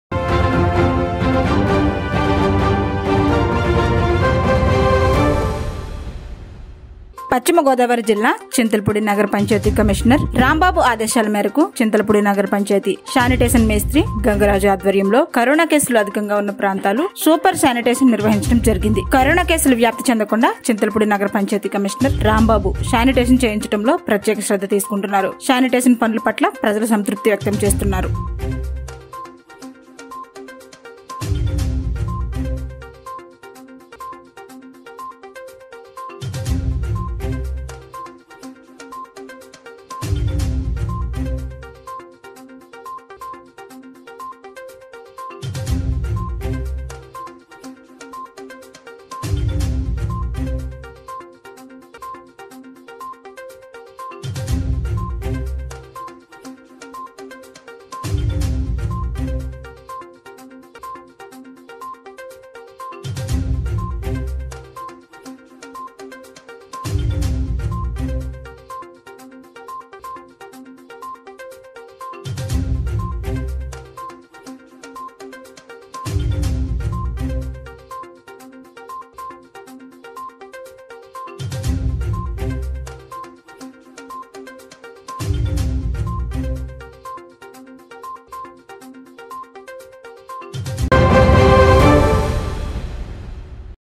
పశ్చిమగోదావరి జిల్లా:చింతలపూడి.కరోనా కేసులు అధికంగా ఉన్న ప్రాంతాల్లొ,ప్రత్యేక శ్రద్ధ తీసుకుంటున్నారు
పశ్చిమగోదావరి జిల్లా:చింతలపూడి.కరోనా కేసులు అధికంగా ఉన్న ప్రాంతాల్లొ,పంచాయతీ కమిషనర్ రాంబాబు శానిటేషన్ చేయించటంలో ప్రత్యేక శ్రద్ధ తీసుకుంటున్నారు.